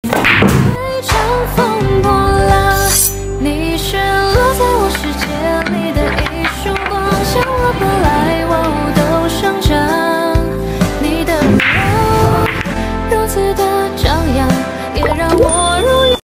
KONLLEN Carbon Fiber Pool Cue Stick Handmade Real Inlay,Ebony Butt,12.3-12.5mm Tip
High-end Turquoise Handmade Inlay Ebony Butt;
Lizard Leather Grip,Sweat-absorbent and non-slip, mature temperament;
The joint is 3/8*8 teeth,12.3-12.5mm Sea-eye Tip，weight is adjustable (you can buy the weight bolts in another link);
Send with a wooden original pool cue case and extension;